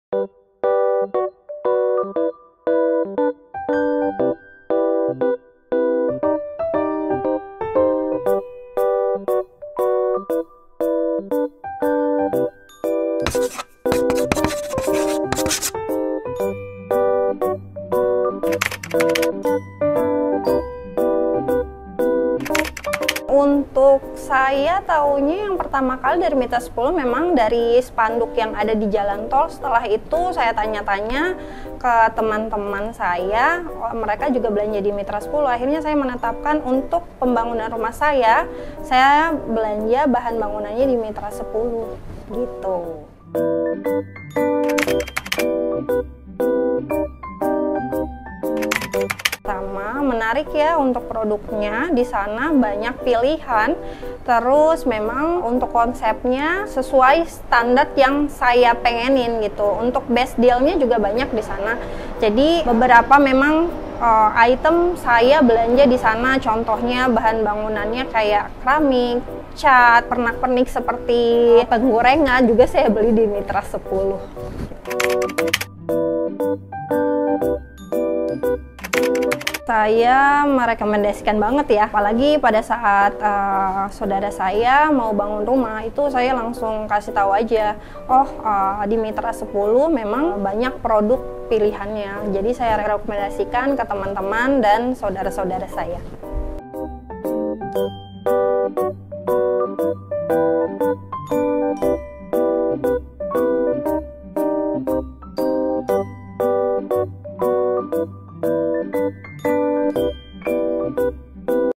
Cerita Sahabat Mitra10 - Kak Meidy | Pilihan Cat Dinting Bikin Ruang Nyaman
Berawal dari melihat spanduk Mitra10 di jalan tol, Kak Meidy akhirnya cari tahu, "apa sih Mitra10?". Sampai berujung belanja di Mitra10 untuk keperluan pembangunan kediaman beliau, karena banyak pilihan juga.

Mulai dari cat, keramik, hingga penggorengan ada loh di Supermarket Bahan Bangunan Mitra10. Yuk, buat Sahabat Mitra10 yang mau renovasi atau bangun rumah, buruan deh ke Mitra10 terdekat!